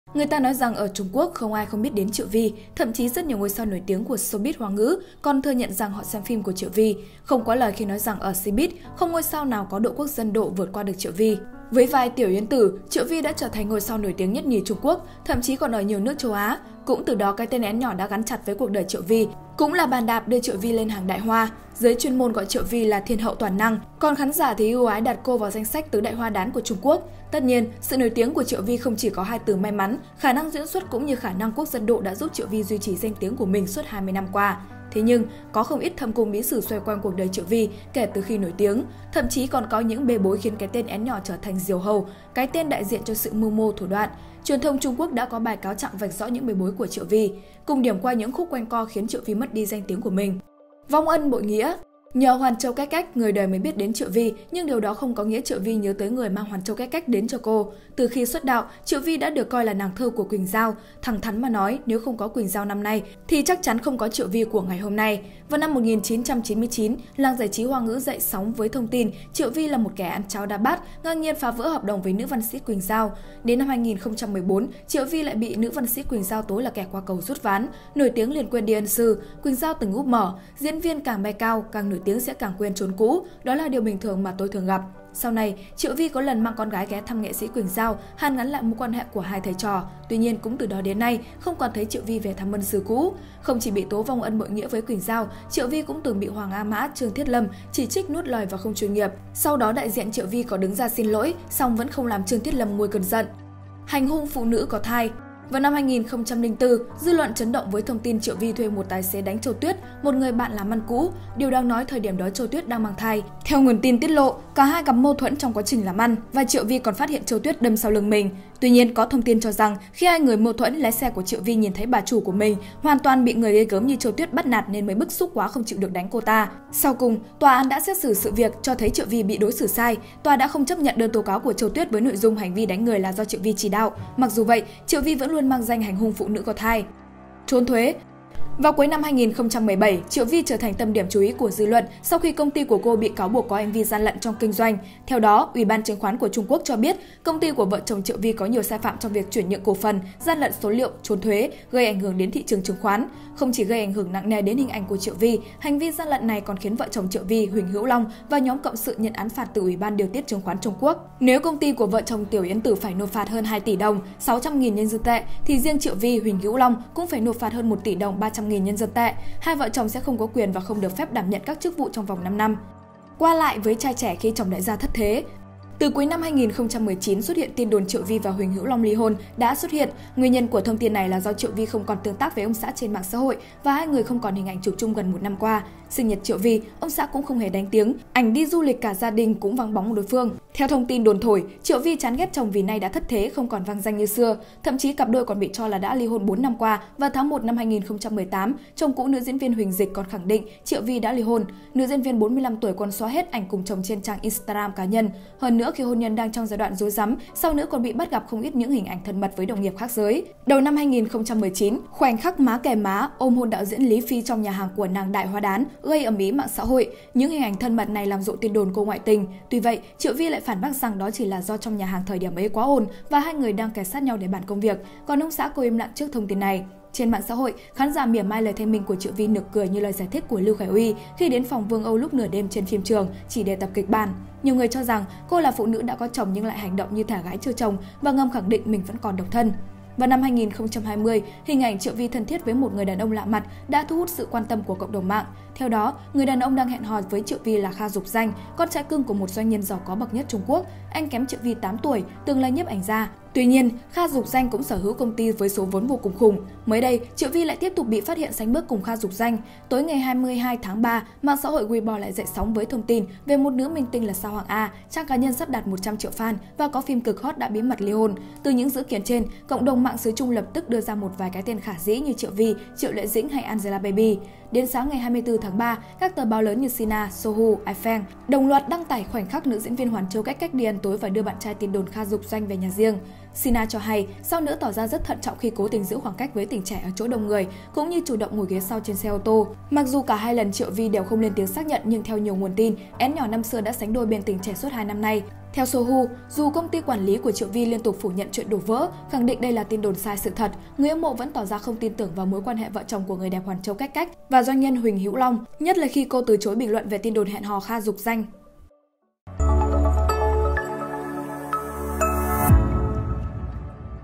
Triệu Vy và những “thâm cung bí sử” xoay quanh “Én nhỏ” suốt 2 thập kỷ
Người ta nói rằng ở Trung Quốc không ai không biết Triệu Vy. Thậm chí, rất nhiều ngôi sao nổi tiếng của showbiz Hoa ngữ còn thừa nhận rằng họ xem phim của Triệu Vy. Không quá lời khi nói rằng ở Cbiz không ngôi sao nào có độ "Quốc dân độ" vượt qua được Triệu Vy.